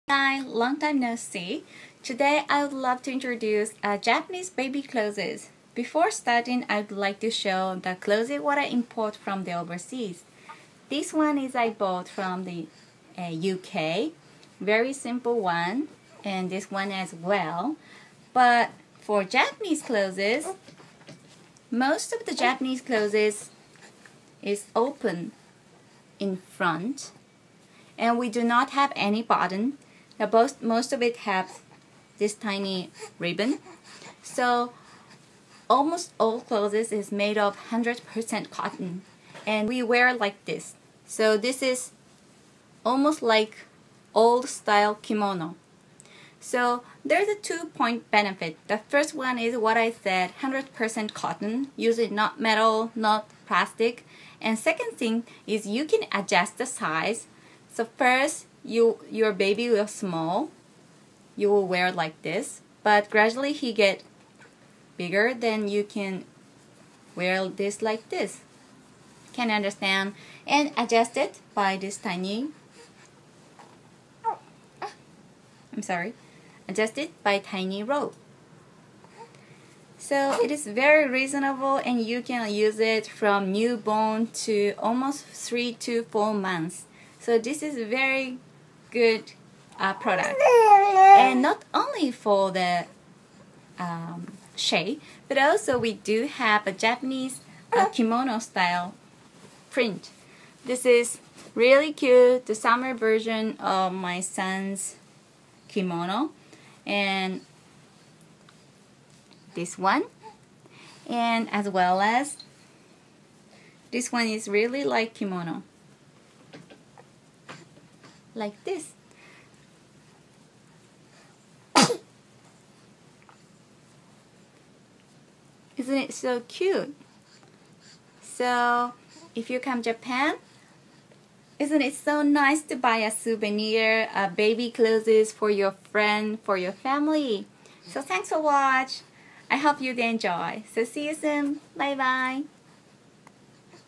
Japanese baby clothes
Have you ever seen Japanese baby clothes? I think there are couples of benefit which is unique to Japanese one. If your country have any cool baby item, please let me know!